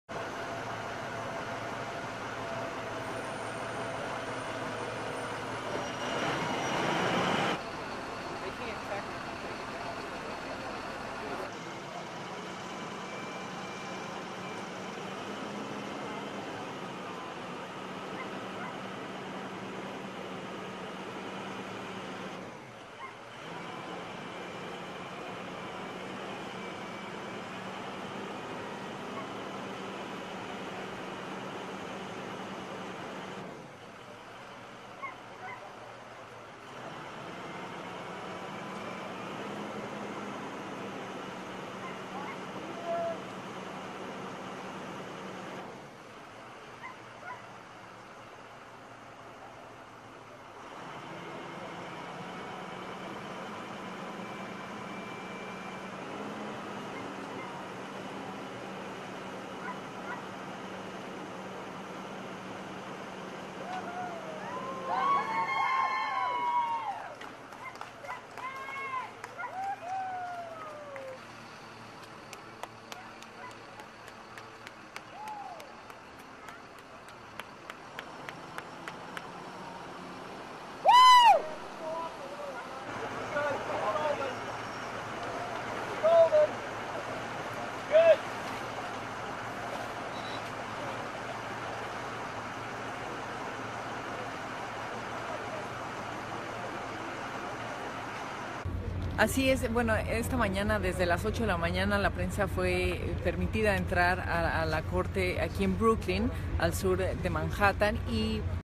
Cubren estatua confederada en Charlottesville | Noticias | Noticias Telemundo
Video oficial de Noticias Telemundo. La medida se produce después de la violencia racial tras una marcha de supremacistas en contra de que la retiraran.